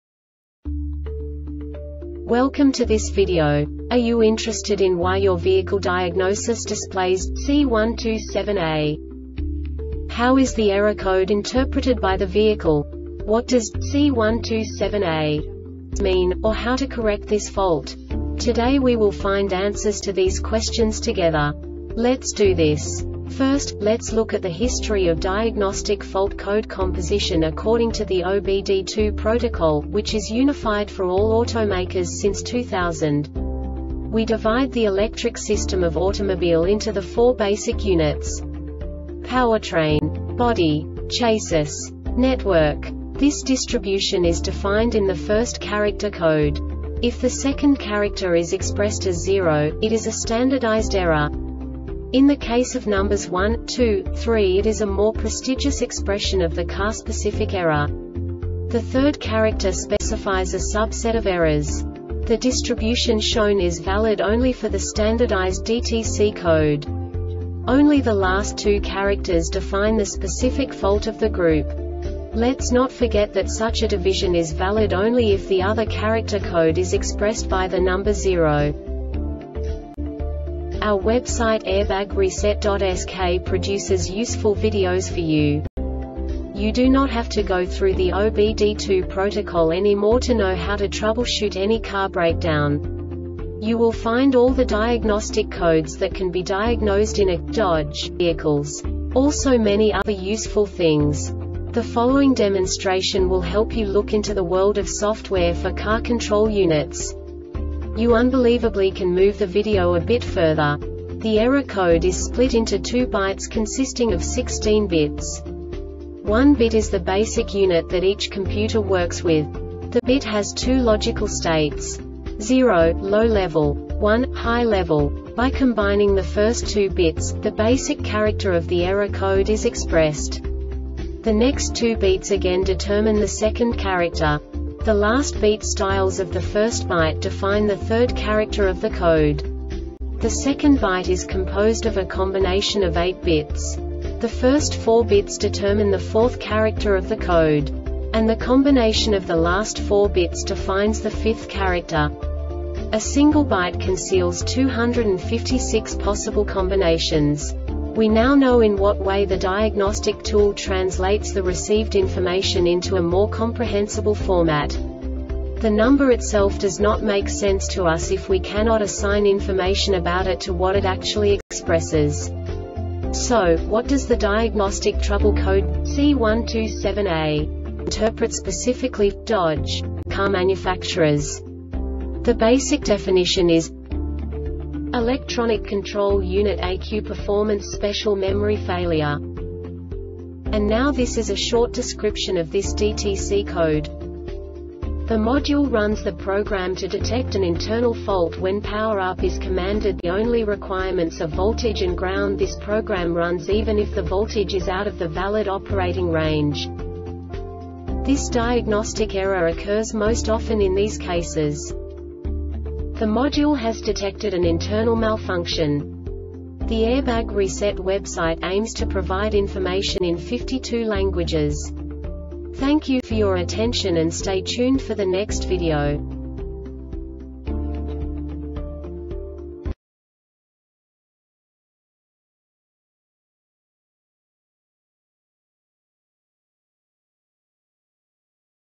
DTC Dodge C127A Short Explanation
Contents:
0:21 Basic DTC analysis according to OBD2 protocol standard.
1:48 Insight into programming
2:58 A diagnostically understandable description of the Diagnostic Trouble Code
3:05 Basic error definition

Make:
Dodge
Code:
C127A
Definition:
Electronic Control Unit (ECU) Performance Special Memory Failure
Description: